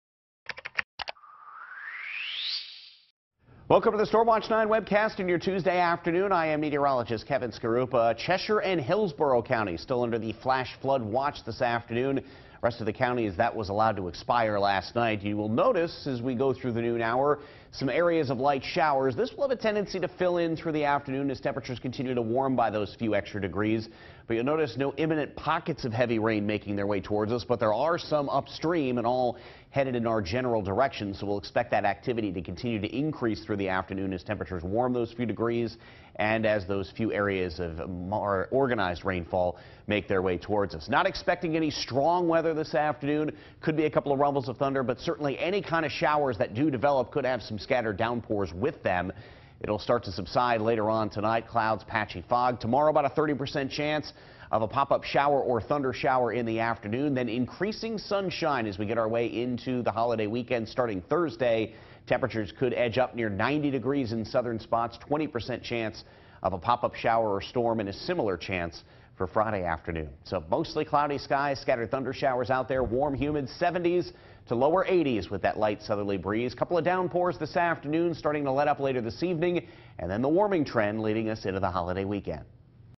Warming trend expected for Fourth of July
Kevin Skarupa has the latest forecast.